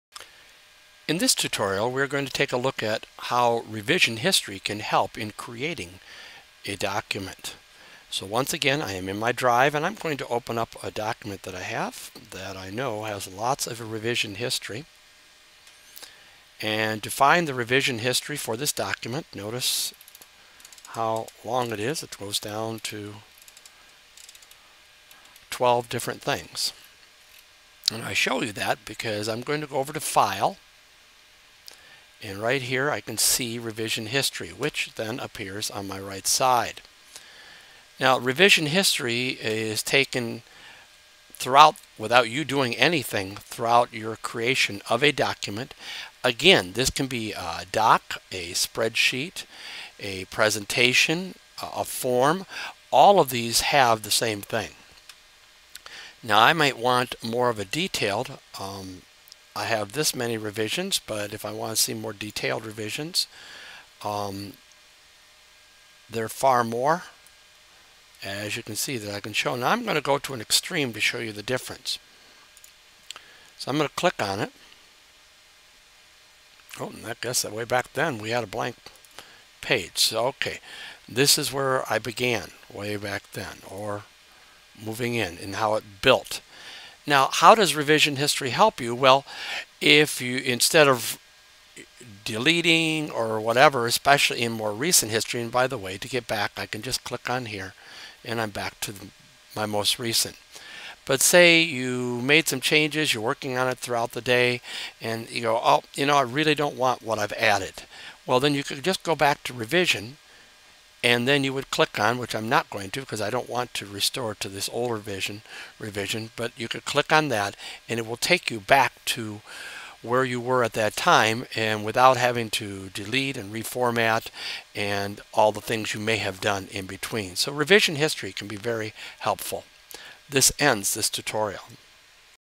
Using Revision History in Google Apps
This is another in a series of training tutorials dealing with Google Apps for Education.